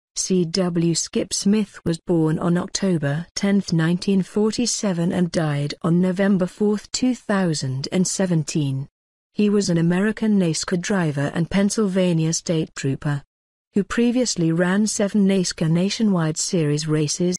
American NASCAR driver C W Smith Died at 70
C.W. "Skip" Smith was born on October 10, 1947 and died on November 4, 2017. He was an American NASCAR driver and Pennsylvania state trooper. who previously ran 7 NASCAR Nationwide Series races .